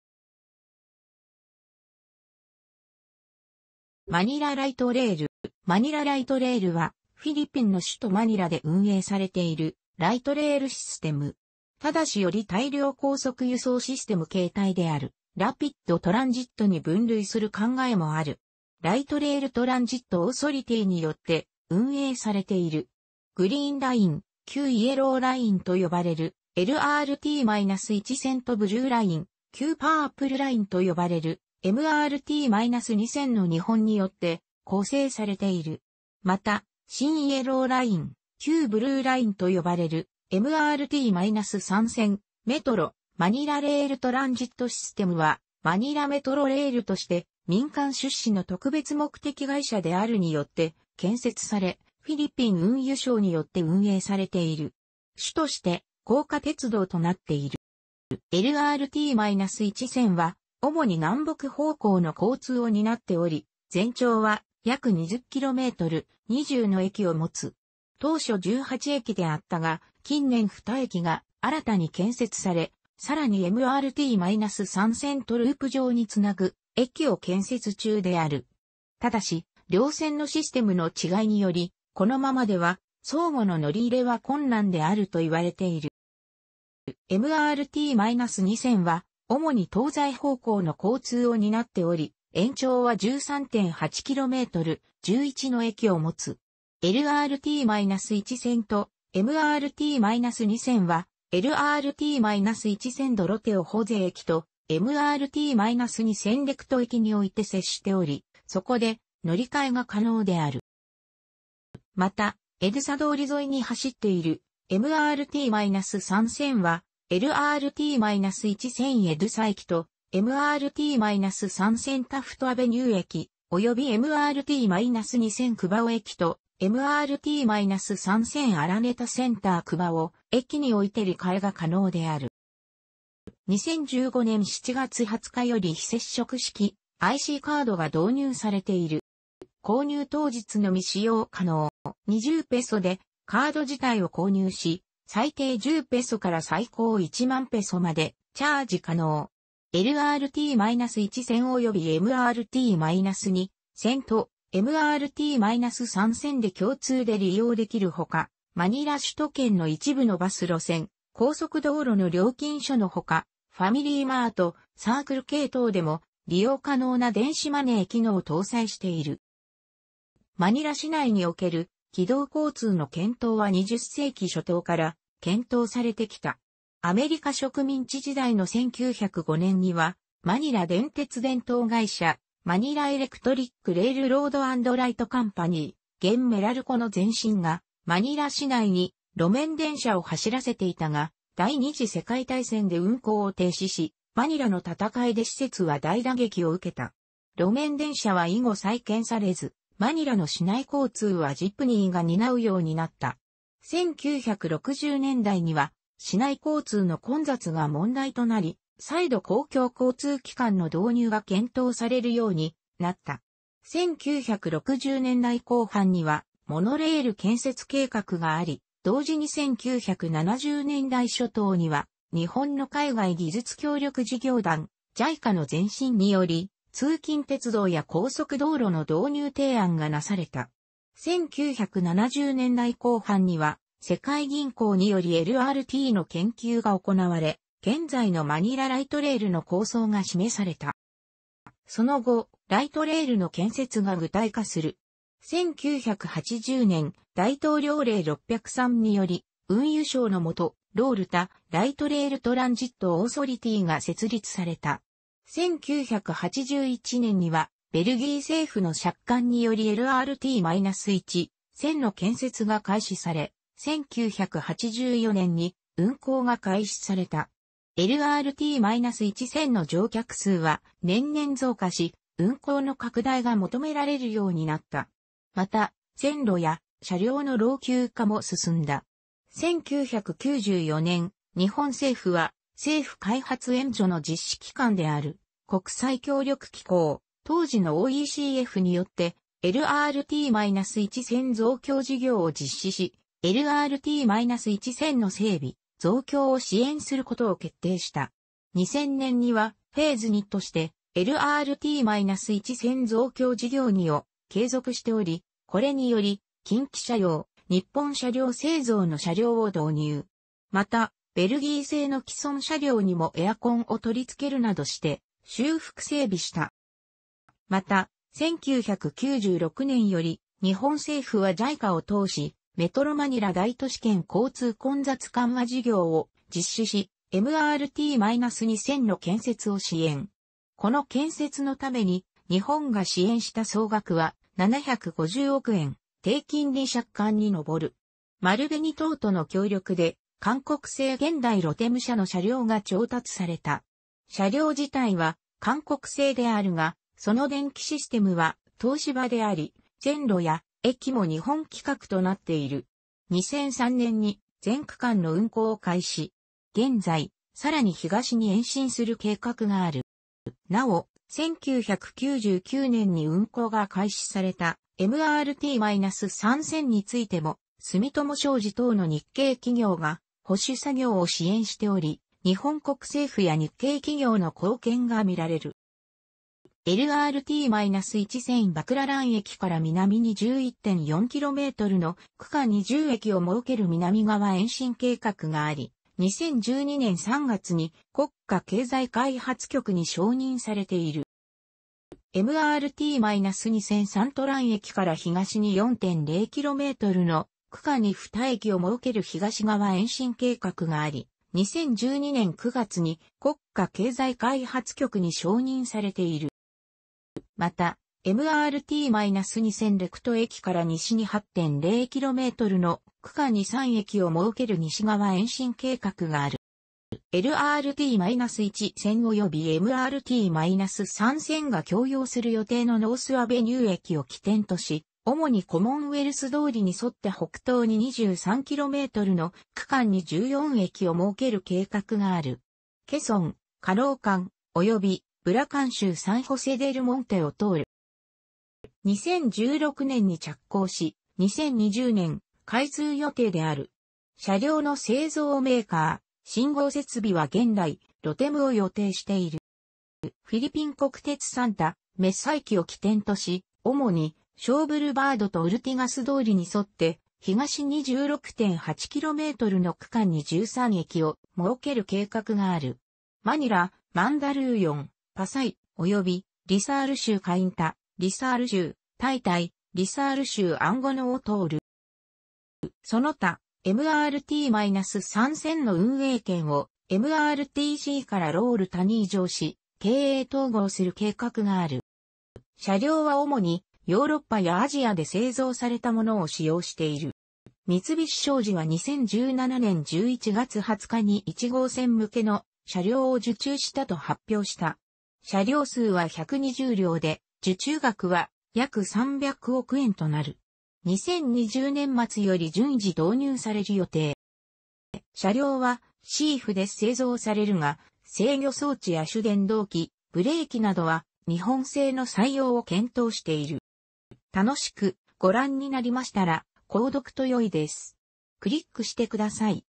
マニラ・ライトレール
マニラ・ライトレール (Manila Light Rail Transit System, Manila LRT) はフィリピンの首都マニラで運営されているライトレールシステム。ただしより大量高速輸送システム形態であるラピッド・トランジットに分類する考えもある。Light Rail Transit Authority (LRTA) によって運営されている。グリーンライン（旧イエローライン）と呼ばれるLRT-1線とブルーライン（旧パープルライン）と呼ばれるMRT-2線の2本によって構成されている。また、新イエローライン（旧ブルーライン）と呼ばれるMRT-3線（Metro Manila Rail Transit System) はマニラ・メトロレールとして、民間出資の特別目的会社である (MRTC) によって建設され、フィリピン運輸省によって運営されている。主として高架鉄道となっている。

LRT-1線は、主に南北方向の交通を担っており、全長は約20km、20の駅を持つ。当初18駅であったが、近年2駅が新たに建設され、さらにMRT-3線とループ状に繋ぐ駅を建設中である。ただし、両線のシステムの違いにより、このままでは相互の乗り入れは困難であると言われている。

MRT-2線は、主に東西方向の交通を担っており、延長は13.8km、11の駅を持つ。LRT-1線とMRT-2線は、LRT-1線Doroteo Jose駅とMRT-2線Recto駅において接しており、そこで乗換えが可能である。

また、エドゥサ通り沿いに走っているMRT-3線は、LRT-1線エドゥサ駅 (EDSA) とMRT-3線タフト・アベニュー駅、及びMRT-2線クバオ駅とMRT-3線アラネタ・センター-クバオ駅において乗り換えが可能である。

2015年7月20日より非接触式ICカードが導入されている。

購入当日のみ使用可能。

20ペソでカード自体を購入し、最低10ペソから最高10,000ペソまでチャージ可能。
LRT-1線およびMRT-2線とMRT-3線で共通で利用できるほか、マニラ首都圏の一部のバス路線、高速道路の料金所のほか、ファミリーマート、サークルK等でも利用可能な電子マネー機能を搭載している。

マニラ市内における軌道交通の検討は20世紀初頭から検討されてきた。アメリカ植民地時代の1905年にはマニラ電鉄電灯会社（Manila Electric Railroad And Light Company、現メラルコの前身）がマニラ市内に路面電車を走らせていたが、第二次世界大戦で運行を停止し、マニラの戦いで施設は大打撃を受けた。路面電車は以後再建されず、マニラの市内交通はジプニーが担うようになった。1960年代には市内交通の混雑が問題となり、再度公共交通機関の導入が検討されるようになった。1960年代後半にはモノレール建設計画があり、同時に1970年代初頭には日本の海外技術協力事業団（JICAの前身）により通勤鉄道や高速道路の導入提案がなされた。1970年代後半には世界銀行によりLRTの研究が行われ、現在のマニラ・ライトレールの構想が示された。

その後、ライトレールの建設が具体化する。1980年、大統領令603により、運輸省の下、LRTA（Light Rail Transit Authority）が設立された。1981年には、ベルギー政府の借款によりLRT-1線の建設が開始され、1984年に運行が開始された。LRT-1線の乗客数は、年々増加し、運行の拡大が求められるようになった。また、線路や車両の老朽化も進んだ。1994年、日本政府は、政府開発援助 (ODA) の実施機関である国際協力機構 (JICA) （当時のOECF）によって、LRT-1線増強事業を実施し、LRT-1線の整備・増強を支援することを決定した。2000年には、フェーズ2としてLRT-1線増強事業Ⅱを継続しており、これにより、近畿車輛・日本車輌製造の車両を導入。また、ベルギー製の既存車両にもエアコンを取り付けるなどして、修復整備した。

また、1996年より、日本政府はJICAを通し、メトロマニラ大都市圏交通混雑緩和事業を実施し、MRT-2線の建設を支援。この建設のために日本が支援した総額は、750億円（低金利借款）に上る。丸紅等との協力で、韓国製現代ロテム社の車両が調達された。車両自体は韓国製であるが、その電機システムは東芝であり、線路や駅も日本規格となっ...